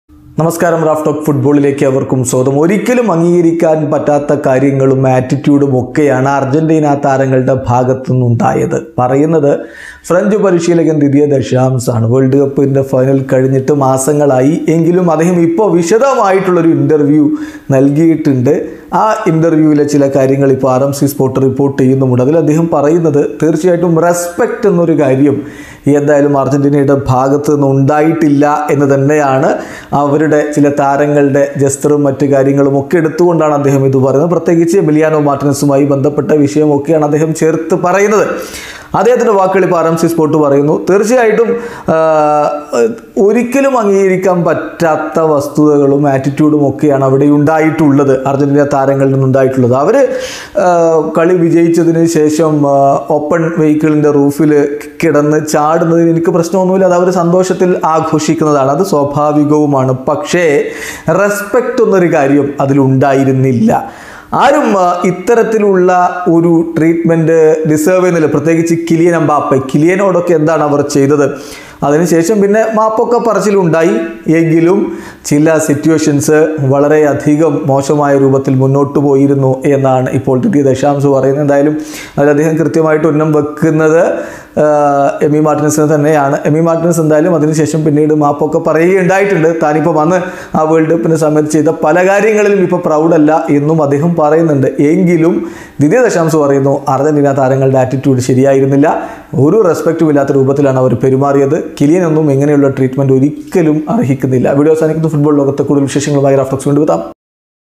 അർജന്റീന താരങ്ങളുടെ ആറ്റിറ്റ്യൂഡ് മോശം, ബഹുമാനമില്ലാത്ത പെരുമാറ്റം: ഫ്രാൻസ് കോച്ച് | Argentina
Deschamps
Football News